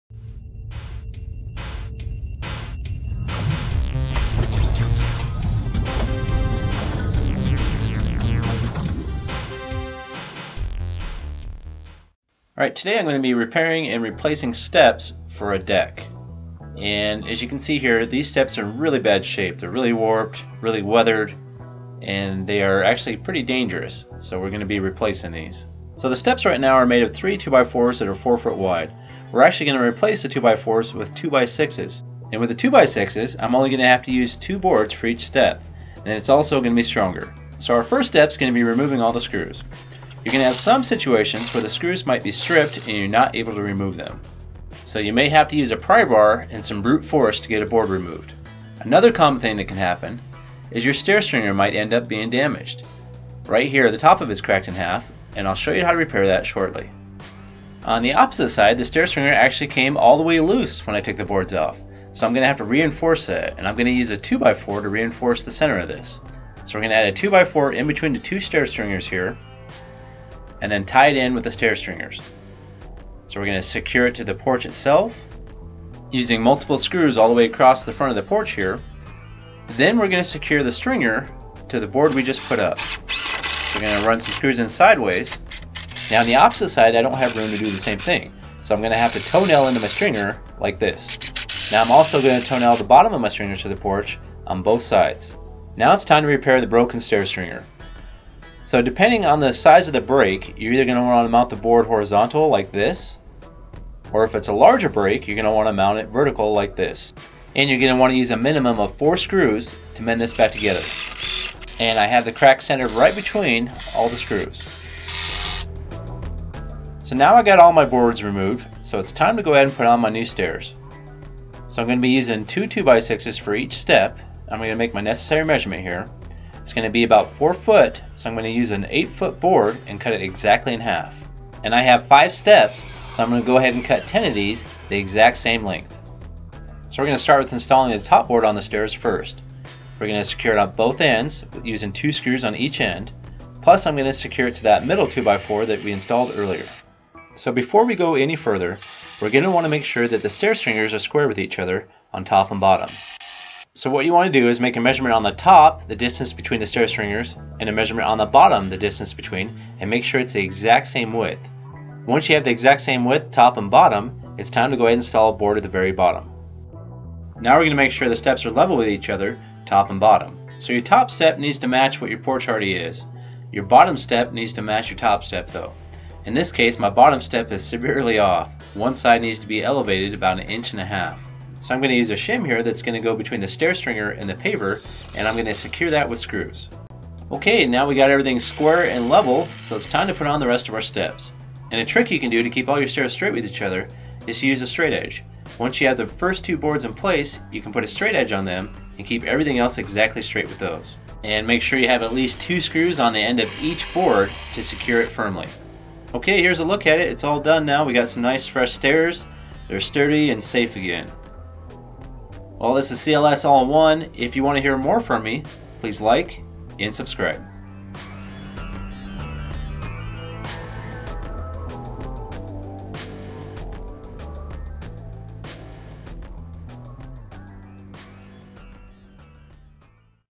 How to repair your porch steps DIY Home Depot materials
How to repair and replace your porch/deck steps and make them like new again.